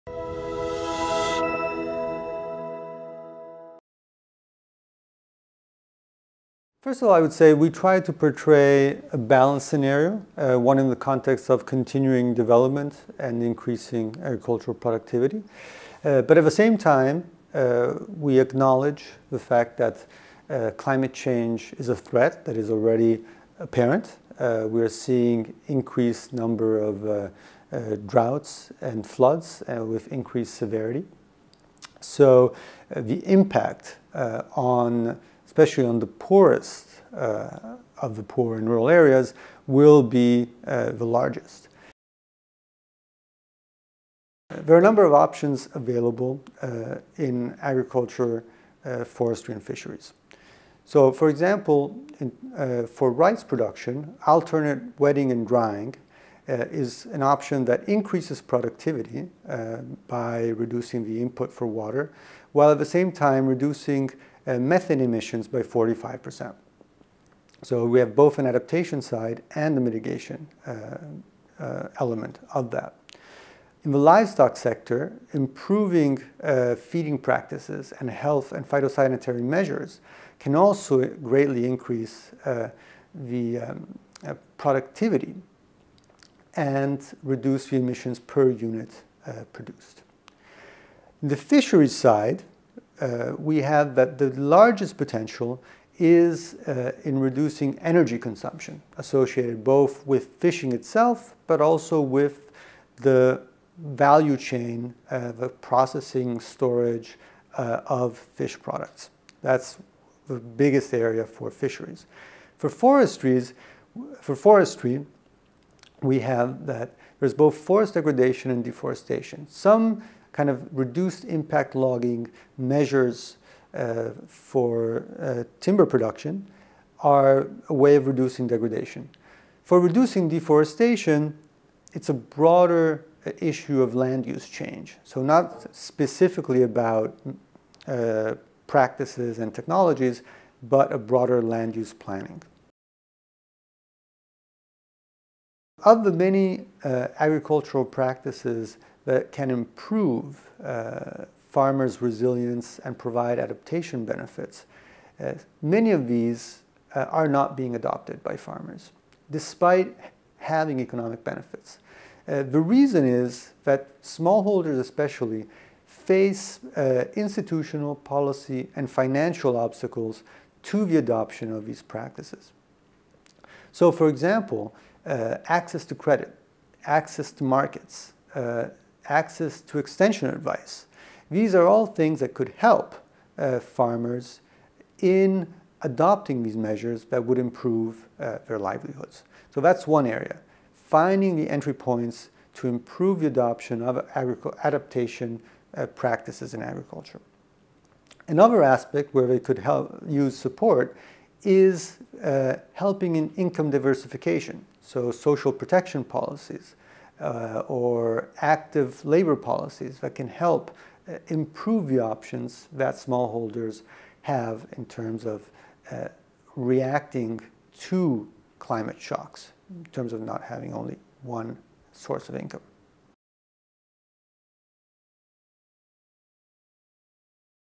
The State of Food and Agriculture 2016 (SOFA)
Agriculture must both contribute more to combating climate change while bracing to overcome its impacts, FAO says in a new report calling for sweeping action to be taken now to ward off the risk it may become impossible for humanity to be fed. Andrea Cattaneo, FAO Senior Economist talks about the latest State of Food and Agriculture report (SOFA), pointing out efficient practices to lower emissions in the various sectors, from agriculture, to fisheries, to forestry, indicating what type of support is needed for smallholders to adapt to climate change. Finally, he underlines the necessity for adaptation and mitigation to go hand in hand.